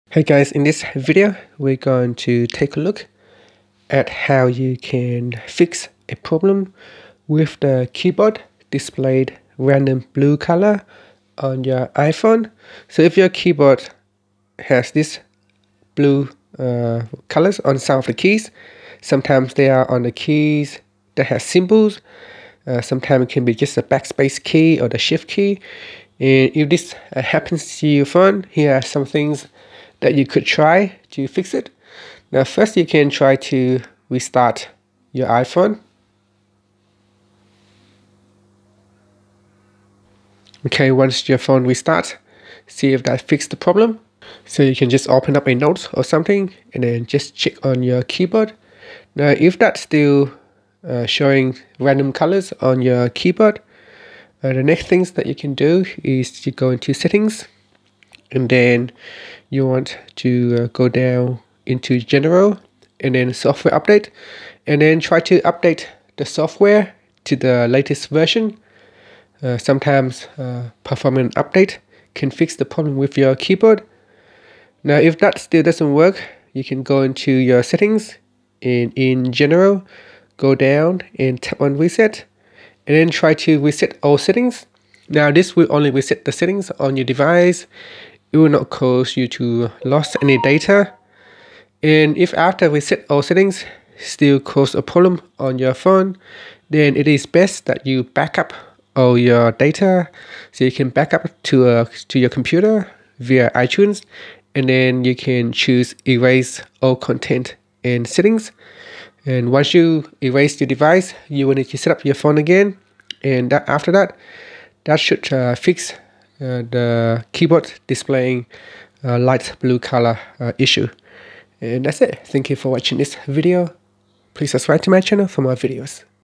Fix iPhone Keyboard Display Random Blue Color Keys
See how you can fix the iPhone keyboard display random blue color keys.